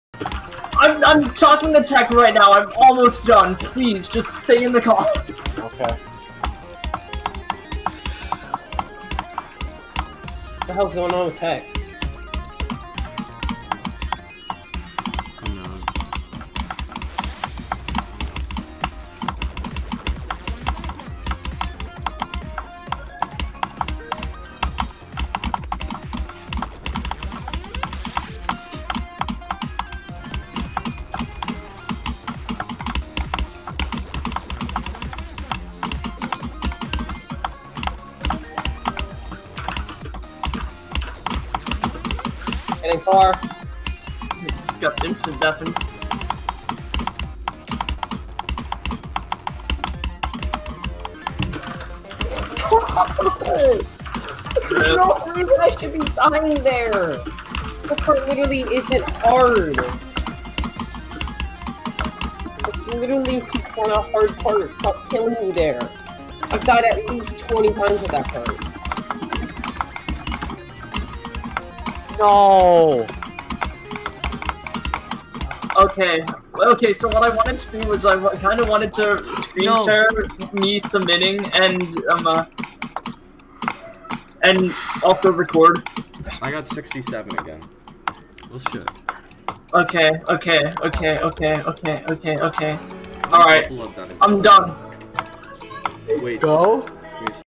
Photovoltaic 67% | Desc
I picked this up at the time i got 85% on Killerzone, so now ive come back to it and progressing fast as hell, im done jumping, this is 26 spots above wander on the legacy list and its very reasonable, its super consistent,it feels meh the worst parts are all torwards  the end
IM FINISHING THIS AND YOU CAN HOLD ME TO IT